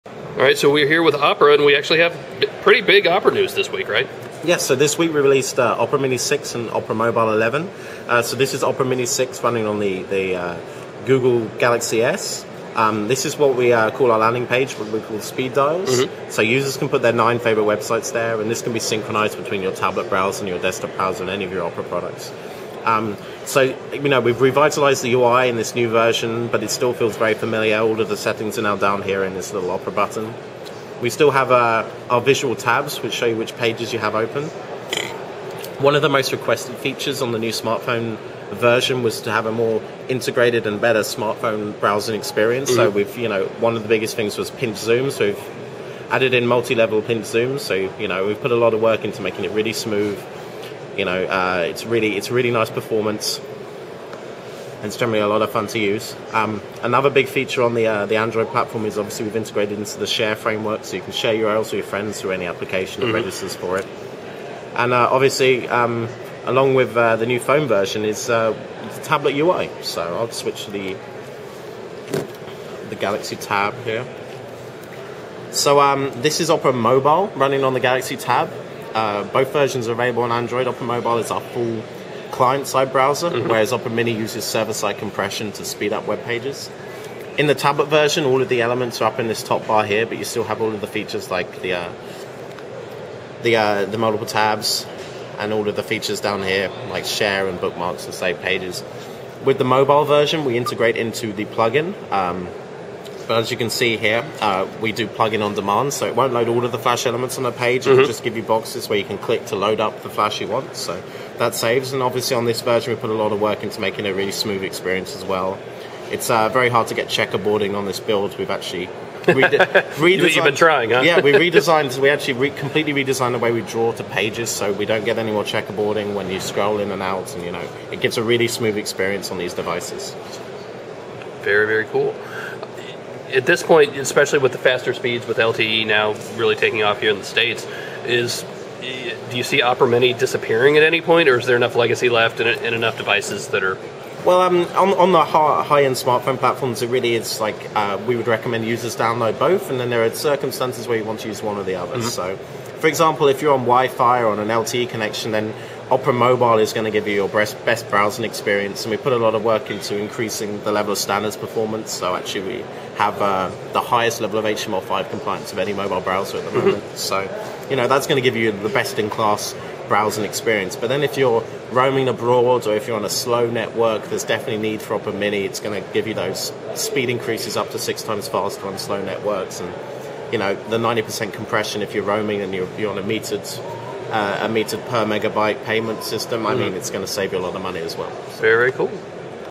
Opera Mobile 6 and Opera Mini 7 Android browsers
Phil sits down with the folks from Opera for a look at Opera Mini 6 and Opera Mobile 11 Android browsers.